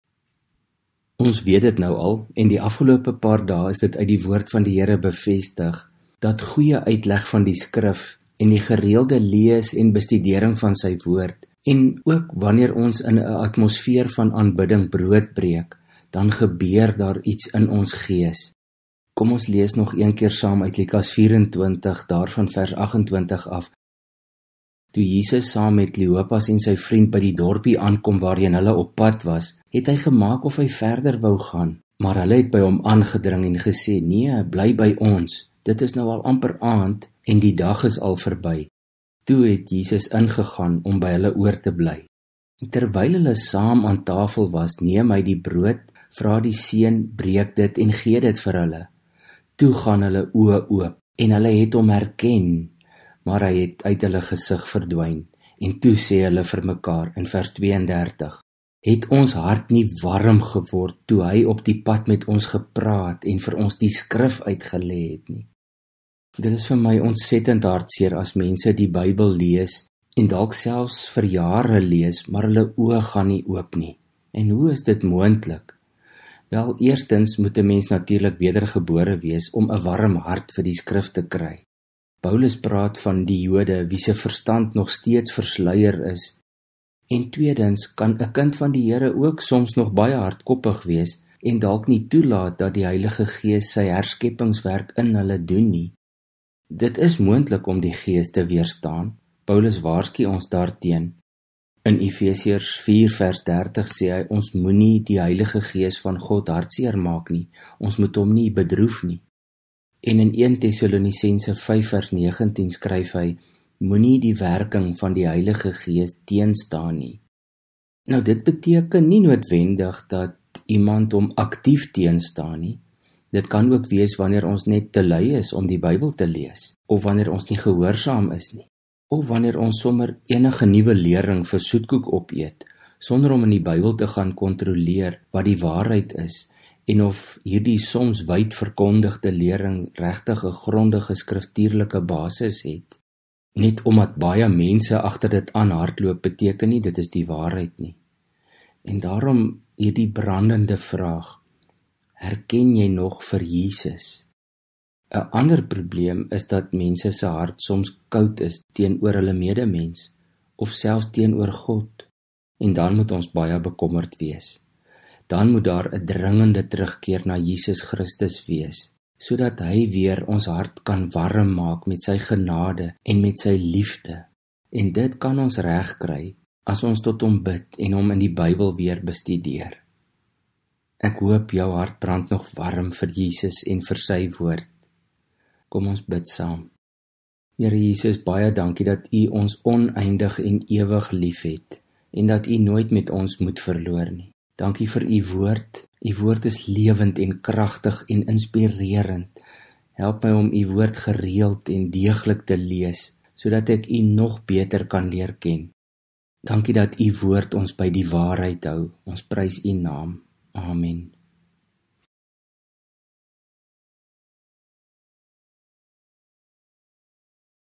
'n Warm hart - oordenking deur johannes vd merwe (ng kerk alkmaar)
'n Warm hart - oordenking deur johannes vd merwe (ng kerk alkmaar) Luk 24:28-32; Efes 4:30 en 1Tess 5:19